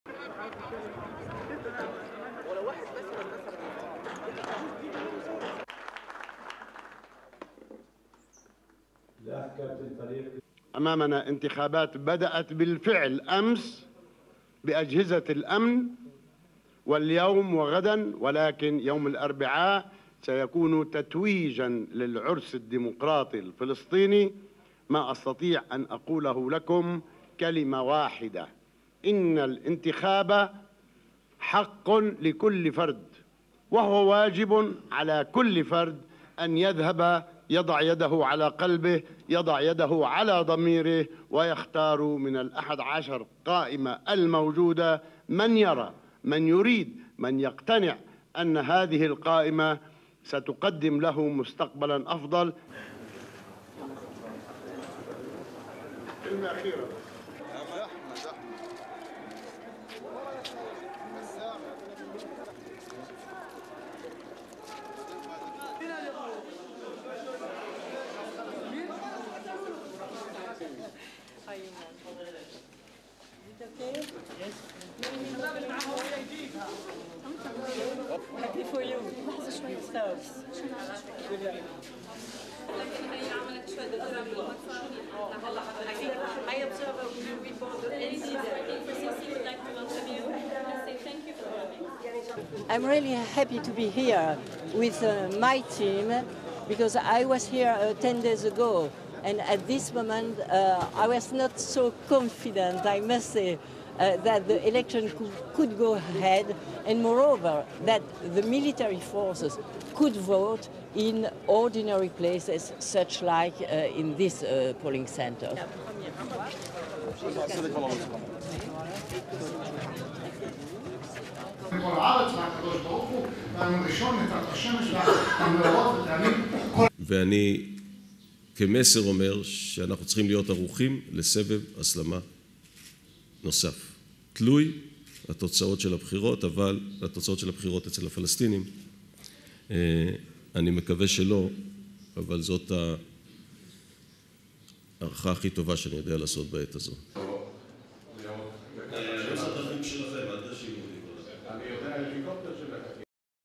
Abbas calls for good turnout at this week's elections
Ramallah, West Bank
1. Palestinian leader Mahmoud Abbas entering ceremony at football stadium
2. Abbas walking to platform
3. SOUNDBITE (Arabic) Palestinian leader Mahmoud Abbas:
"We have elections which started yesterday with the security forces, they will continue today and tomorrow. But on Wednesday we'll be celebrating a democratic "wedding". What I say to you in one sentence is that voting is a right and duty for everyone to go and put his hand on his heart and chose from the 11 lists the one that he believes in, the one he thinks will give him a better future."
4. Wide shot of Abbas

Gaza City, Gaza Strip
5. Palestinian security guard leads EU observers to polling station
6. Véronique de Keyser walks to polling station
7. Various of Véronique de Keyser meeting elections officials in polling station
8. SOUNDBITE: (English) Véronique de Keyser, head of EU observers delegation:
"I am really happy to be here with my team because I was here 10 days ago. And at this moment I was not so confident, I must say, that the elections could go ahead. And moreover that military forces could vote in ordinary places such as like in this polling centre."
9. Wide shot of EU observer in polling centre

Herzliya, Israel
10. Wide shot of Halutz at news conference
11. SOUNDBITE: (Hebrew) Dan Halutz,  Israeli Defence Forces Chief of Staff
"My message is that we have to be prepared for an additional escalation (in violence) which depends on the results of the Palestinian elections. I hope not, but this is what we're assessing at the moment."
12. Wide shot Halutz at  news conference

STORYLINE:

Palestinian leader Mahmoud Abbas on Sunday called on all Palestinians to vote in parliamentary elections, calling it "a right and duty for everyone."

A leading Palestinians official said Abbas' Fatah Party will only form a coalition government with parties willing to negotiate with Israel, and won't offer to include Hamas militants if they don't renounce violence.

Recent polls indicate that Fatah and Hamas are running neck-and-neck ahead of Wednesday's parliament vote.

But Information Minister Nabil Shaath said he was confident Fatah will win enough of the legislature's 132 seats to form a government on its own.

Then it would offer to share power with parties that accept the US-backed "road map" peace plan and recognize Israel he said.

Later on Sunday Véronique de Keyser, the head of the EU election observer team in Gaza, visited a local school there which will act as a polling station for when civilian Palestinians come to vote.

de Keyser said that her team was satisfied from the way the Palestinian Authority was handling the election process.

"I am really happy to be here with my team because I was here 10 days ago. And at this moment I was not so confident, I must say, that the elections could go ahead," she said.

In Israel in the town of Herzliya, Israeli Defence Forces Chief of Staff General Dan Halutz said the country should be prepared for a possible deterioration in security after the results of the Palestinian Authority's election results are announced.

"My message is that we have to be prepared for an additional escalation (in violence) which depends on the results of the Palestinian elections," he told a news conference.